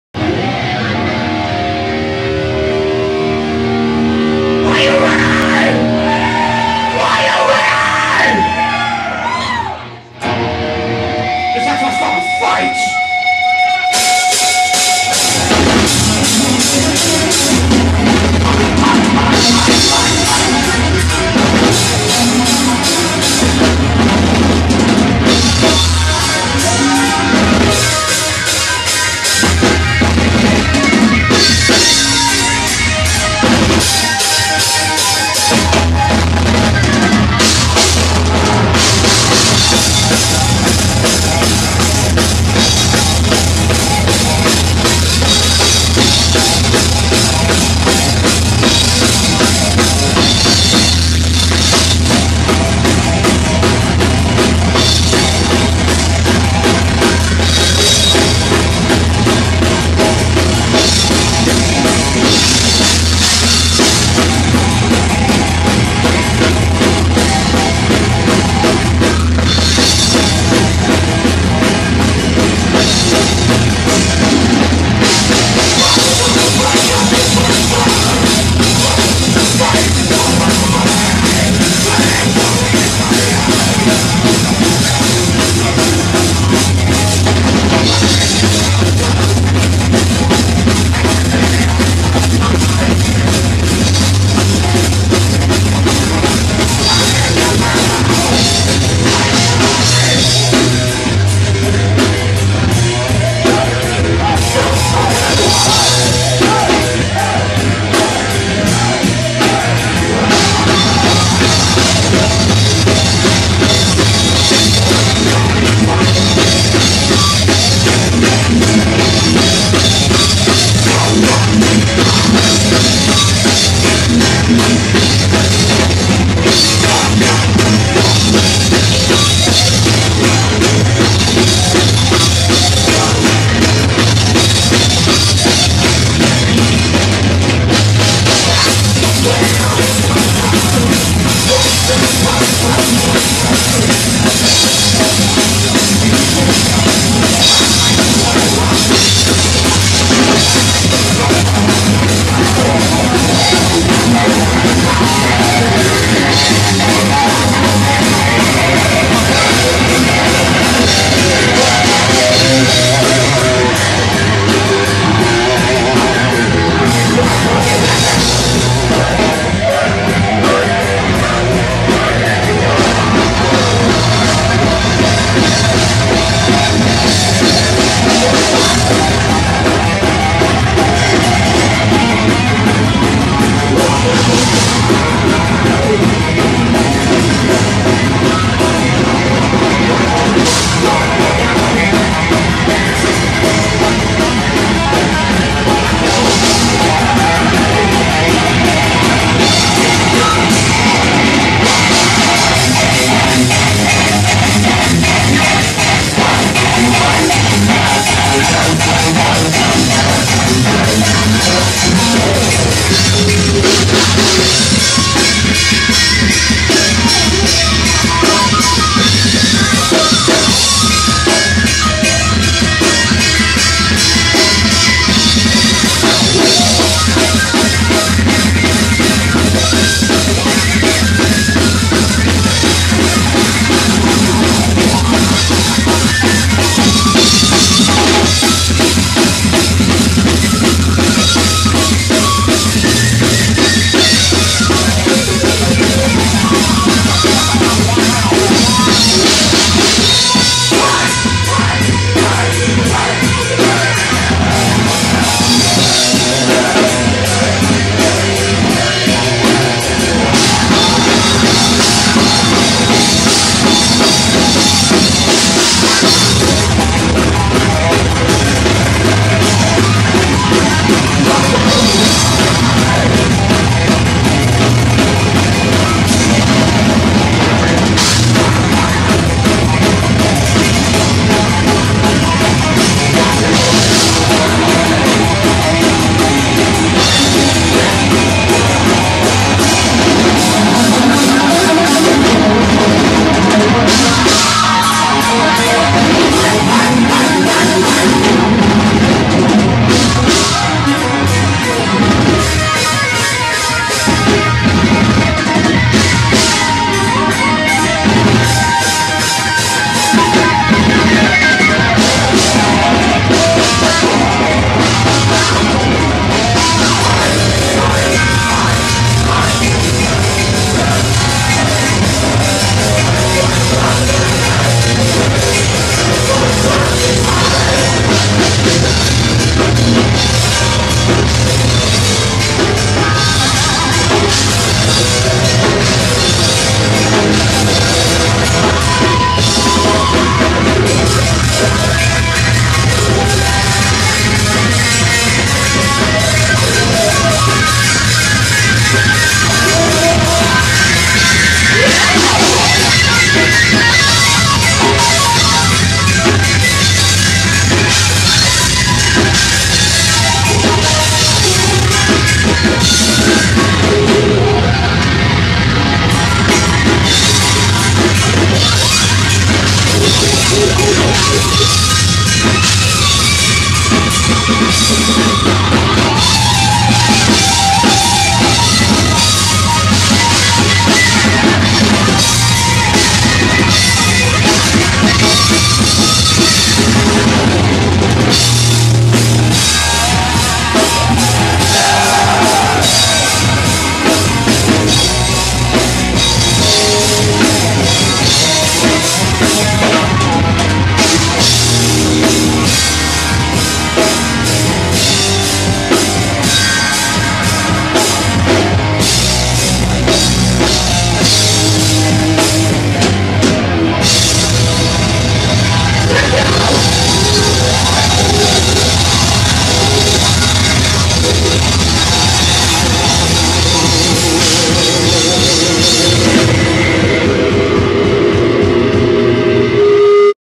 Furnaze Live Fight at the Skullcrusher Dresden
Good times we had at the Skullcrusher last year thanks to our loyal fans in Dresden- you guys rock!